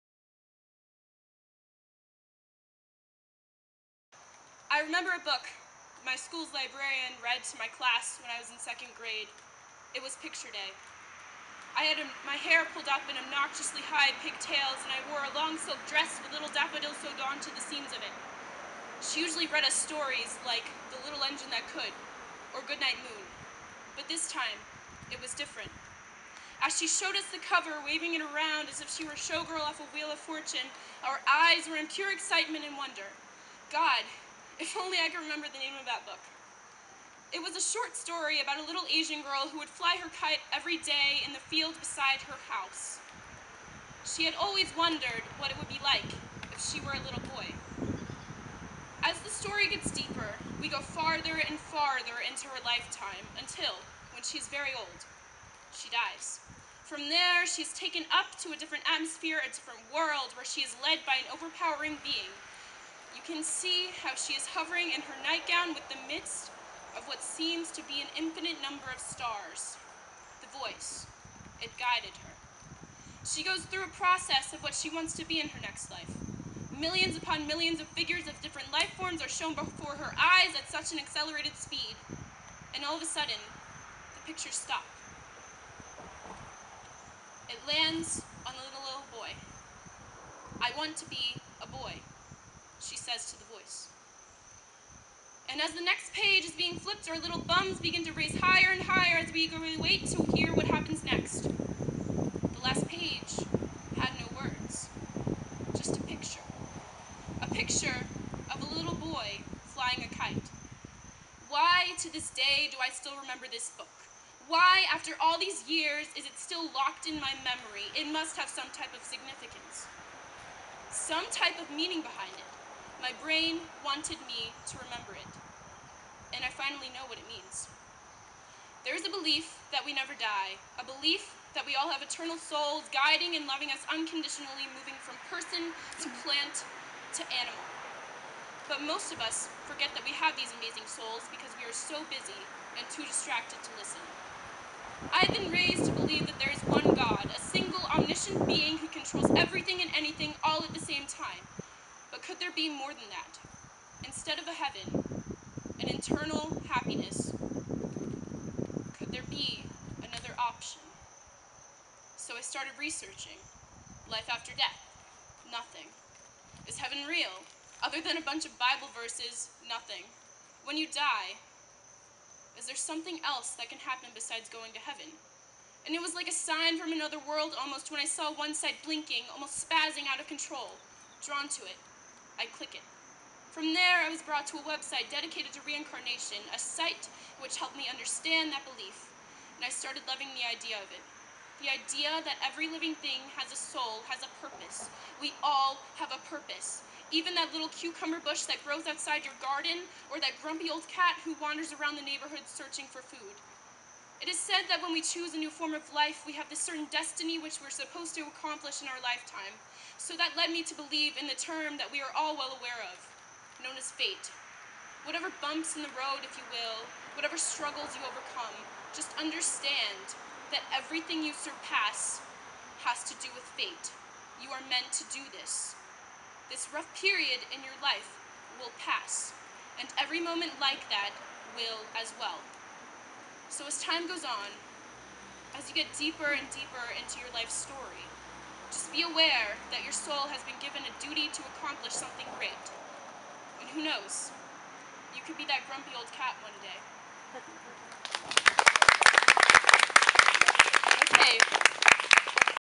Slam Poetry: We All Have Souls at LTPS August 2014
Chloë Bartholomew performs "We All Have Souls" at LTPS August 2014.

EDIT: video misspells "Chloë" as "Chlöe"!